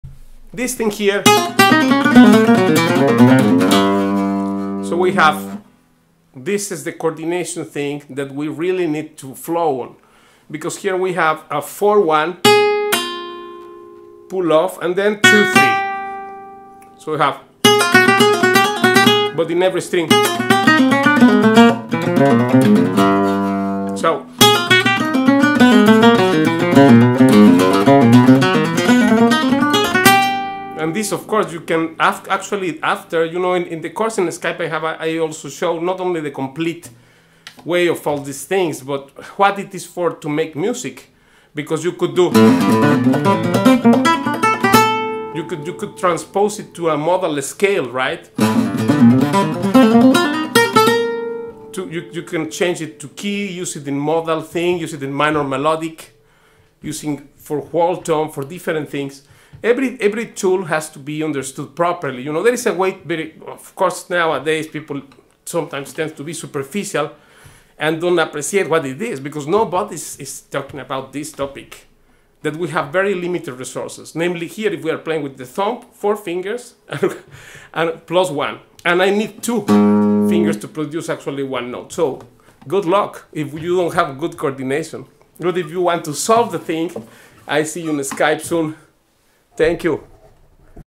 Never show these to your girlfriend n 8 Guitar things U don't find anywhere else but here Ruben Diaz
Some Compositions for guitar;

Real training vs Do it yourself (flamenco guitar) by just watching videos not much can be learned...

This channel exist TO INSPIRE YOU on Paco de Lucia's style

Today there is absolutely zero new stuff in flamenco, nothing which Paco has not done 30 years ago.

A very useful topic by the great Kenny Werner

^Ex.1/ 00:01
Ex.2/ 01:52
Ex.3/ 09:52
Ex.4/ 04:38 topic